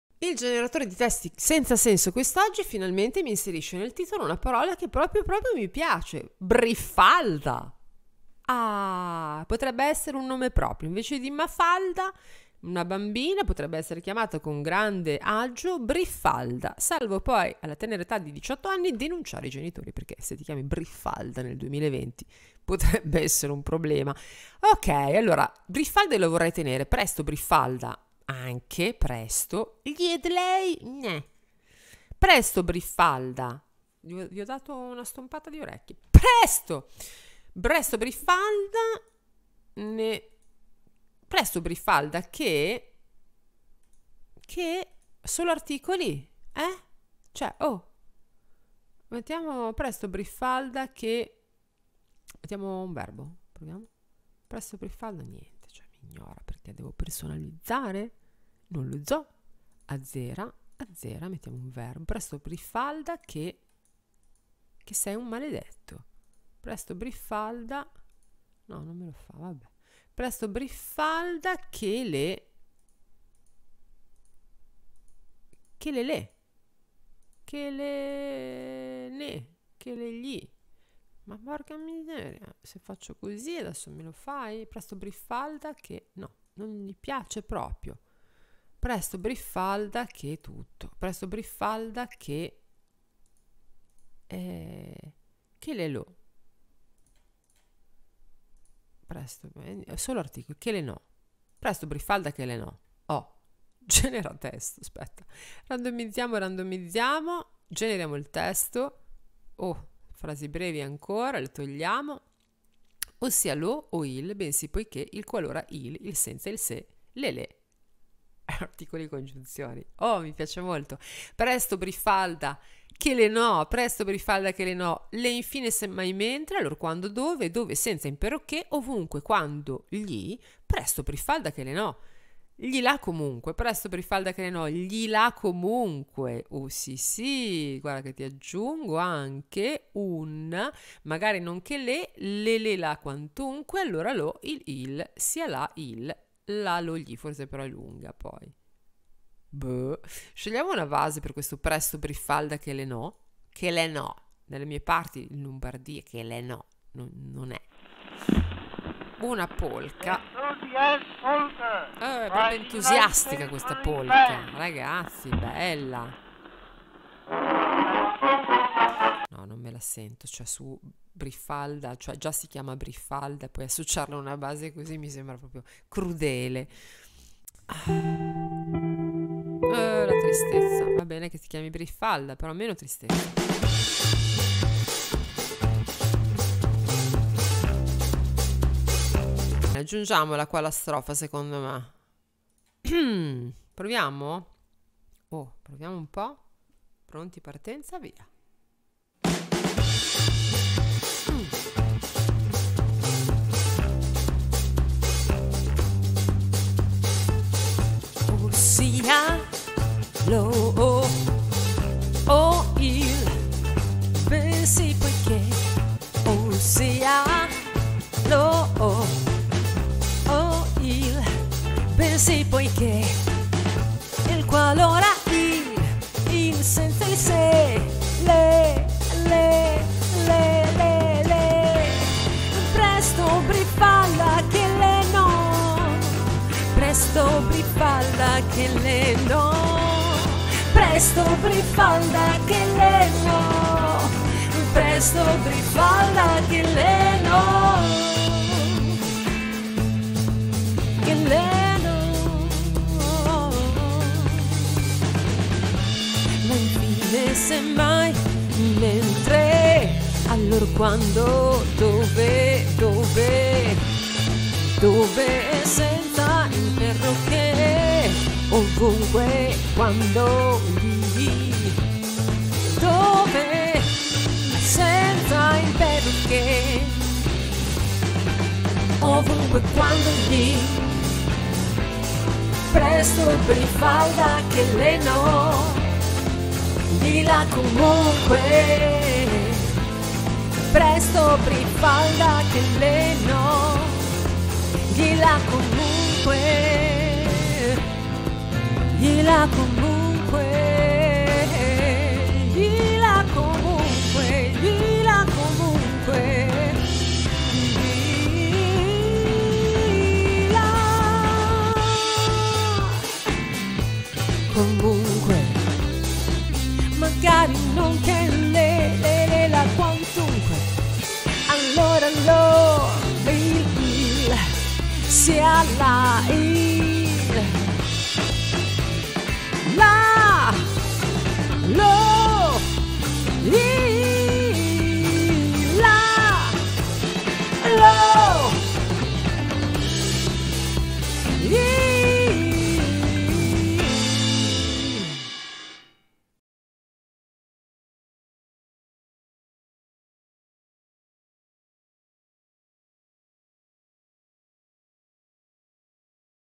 Per la serie "Insulti Creativi": Briffalda
Testi Senza Senso ti permette di dare della "poco di buono" a qualsiasi signorina sfacciata, ma te lo fa fare sempre con una certa classe.